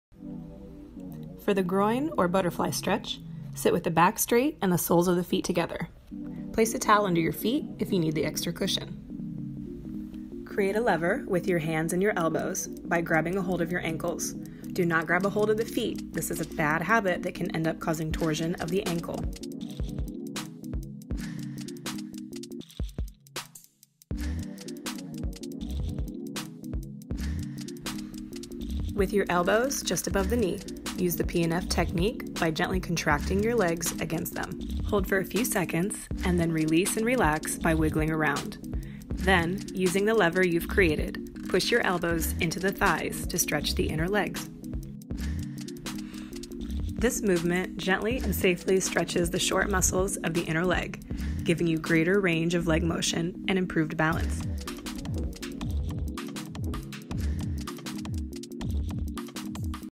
Groin/Butterfly Stretch tips | Essentrics Techniques
.Quick video on how we perform the groin/butterfly stretch in an Essentrics class. After year of ballroom dancing I found that I had developed very tight inner leg and groin muscles. To help rebalancec my legs and hips I train with Essentrics and this is one of the many helpful movements we do in the full body routines.

Useful tips and modifications:
* Sit on a riser or block if you cannot sit in this position with a straight back.
*Place a towel under your feet for cushioning and to help with ankle alignment if needed.
*Do NOT GRAB YOUR FEET. This can cause torsion in the ankles and overstretch the ligaments and tendons of the feet. Instead grab your ankles.
*Use PNF contract/relax/release method to gain more flexibility and ROM faster.
*Repeat this movement at least 3 times.

Leave questions and comments below so I can respond back.